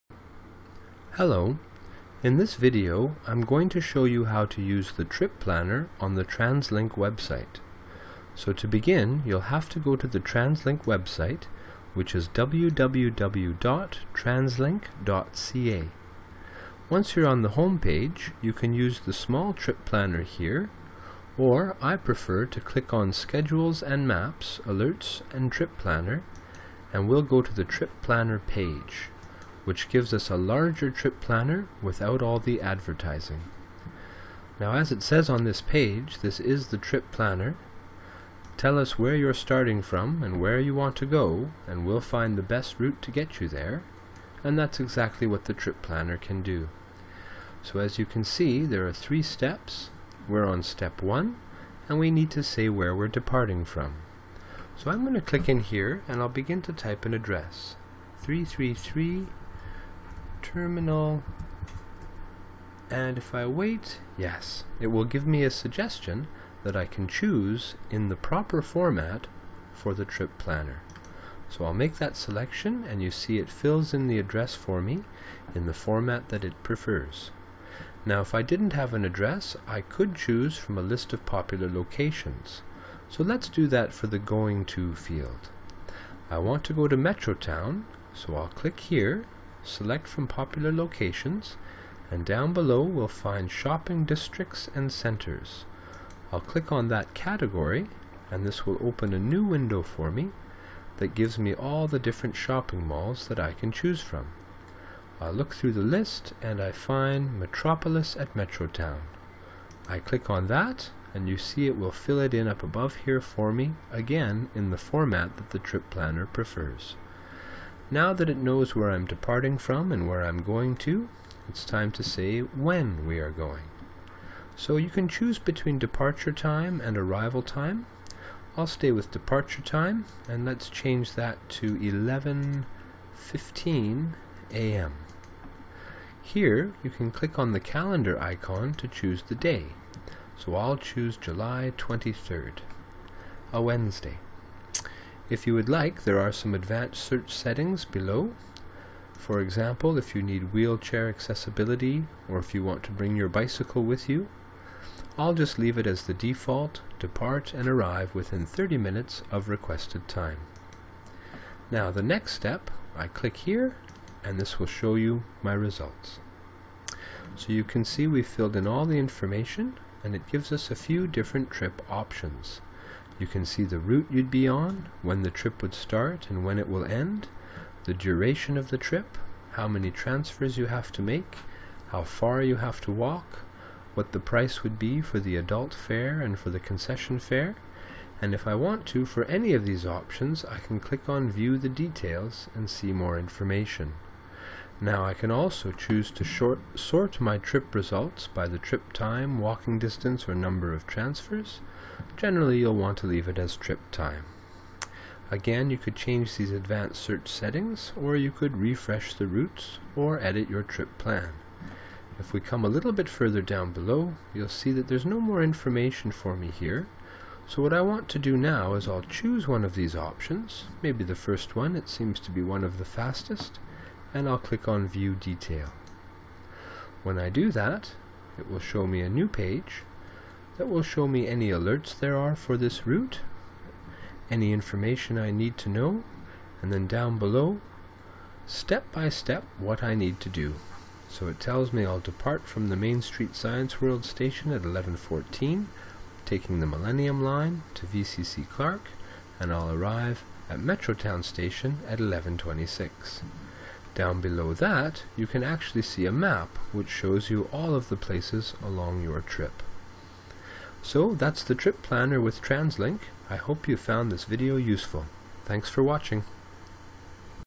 How to Use the Translink Trip Planner
This video will show you how to use the Translink Trip Planner.